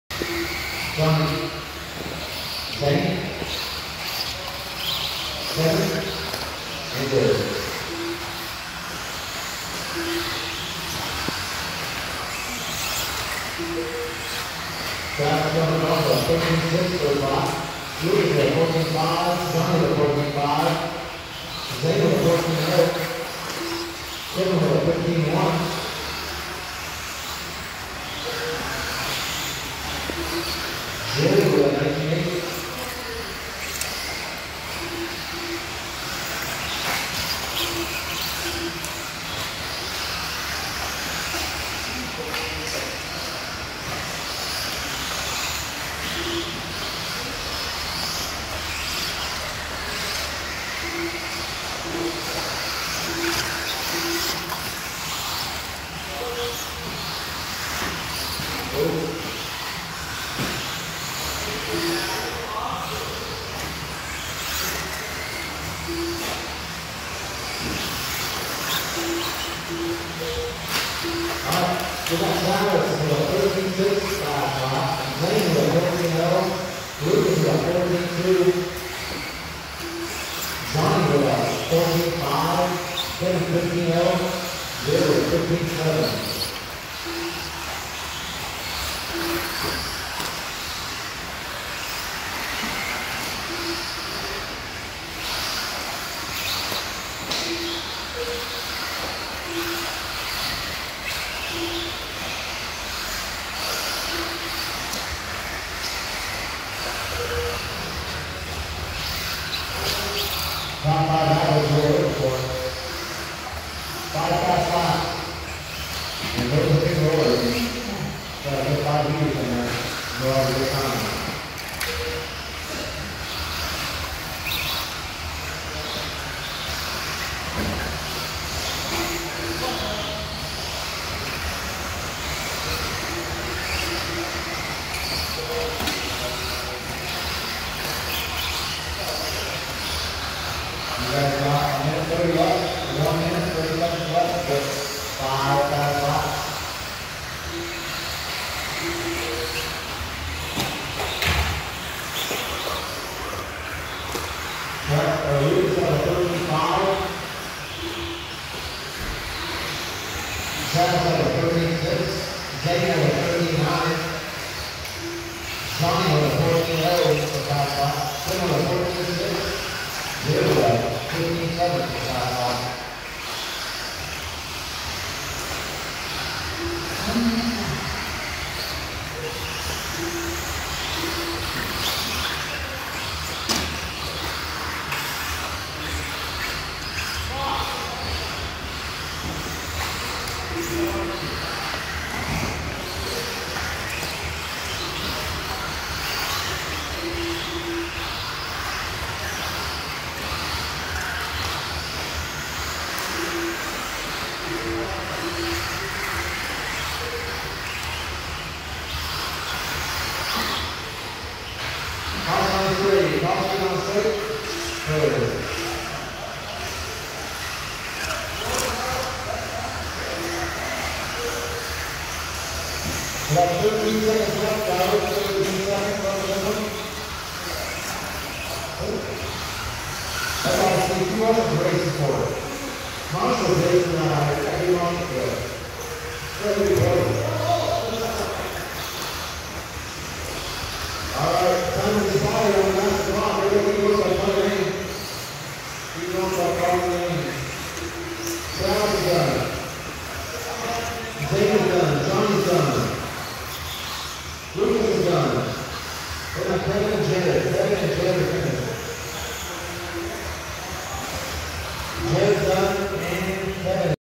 2019 RC Compound Summer Slammer 17.5 2WD Buggy Seeding 2 6-29-19
2019 RC Compound Summer Slammer

This event ran from 6/28-29 at the RC Compound in Des Moines, IA.